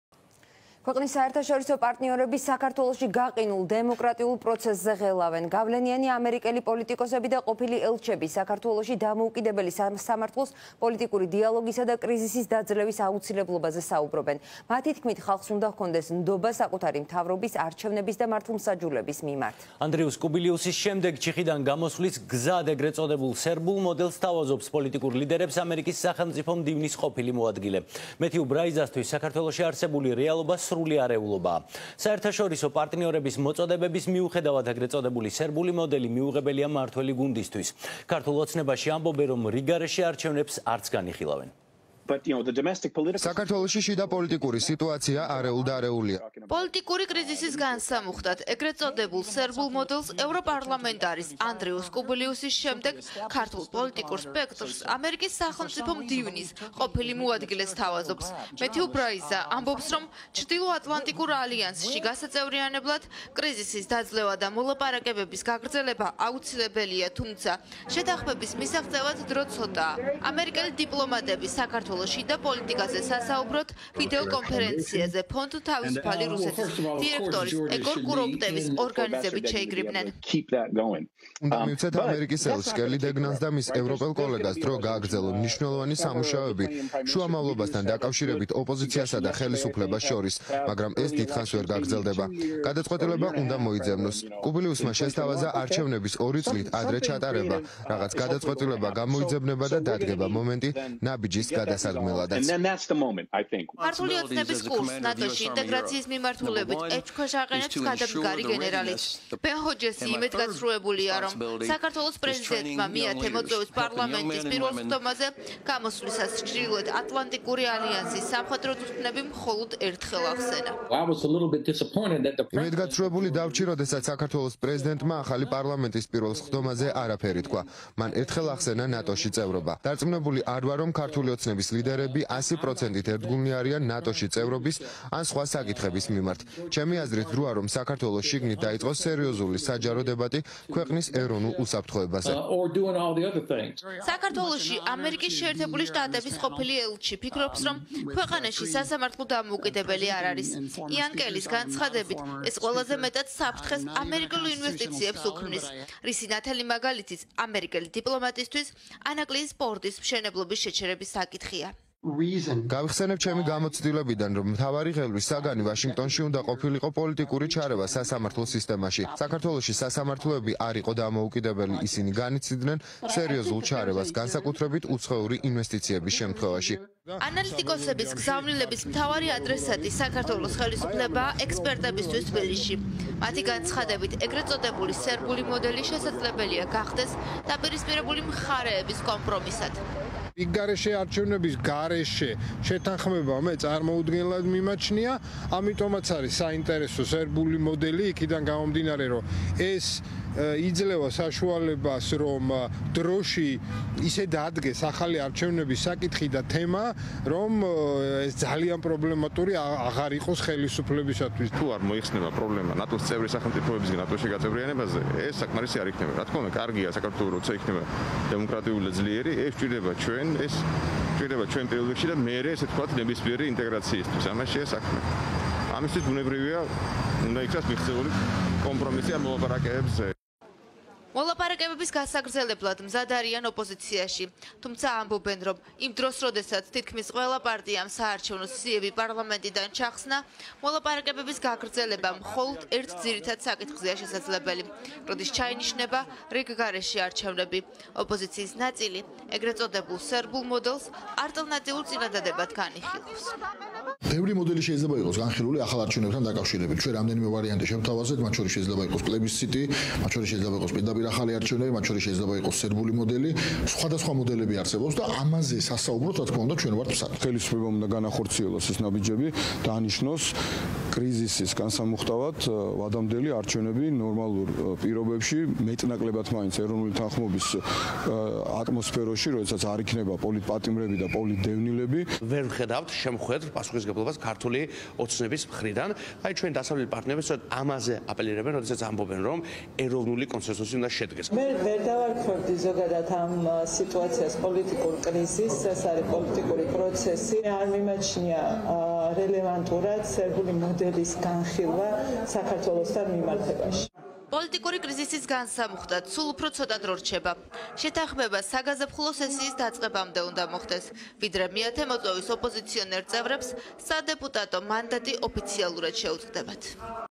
„არეულობა" საქართველოს მთავრობაში
▪️ „არეულობა" საქართველოს მთავრობაში - ქვეყნის საერთაშორისო პარტნიორები საქართველოში გაყინულ დემოკრატიულ პროცესზე ღელავენ